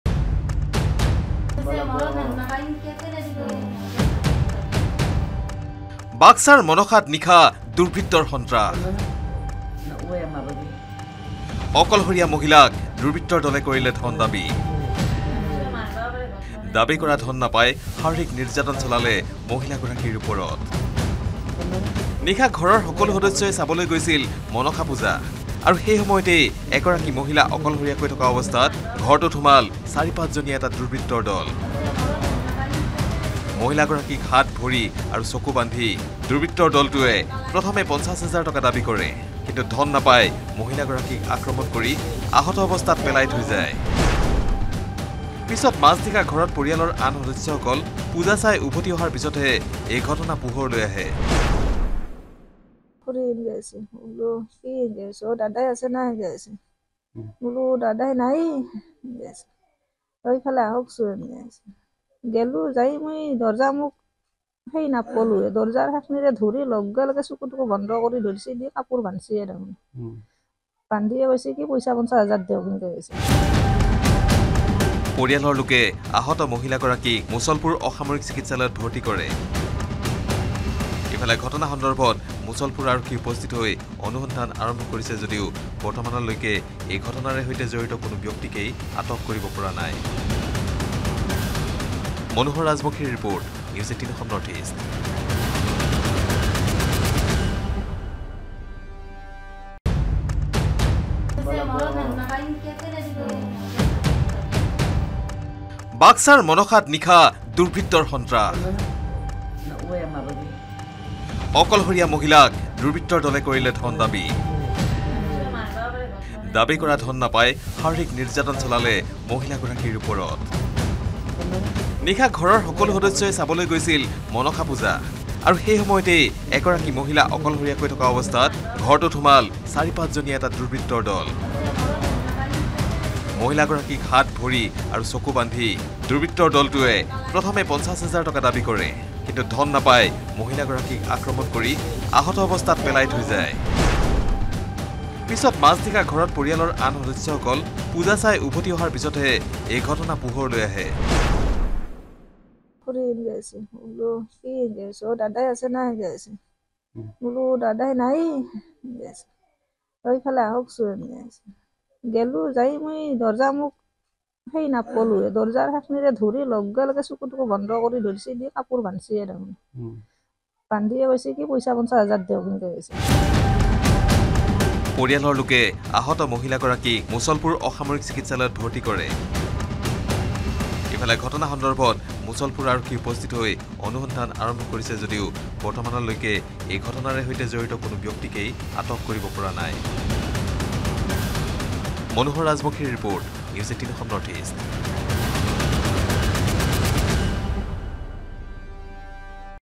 Mushalpur Crime News | অকলশৰীয়া মহিলাক দূৰ্বৃৃত্ত দলৰ ধন দাবী | N18V
Mushalpur Crime News |  বাক্সাৰ মনসাত নিশা চাঞ্চল্যকৰ ঘটনা
অকলশৰীয়া মহিলাক দূৰ্বৃত্বৰ দলৰ ধন দাবী
দাবী কৰা ধন নাপাই শাৰিৰীক নিৰ্যাতন মহিলাগৰাকীক
n18oc_crime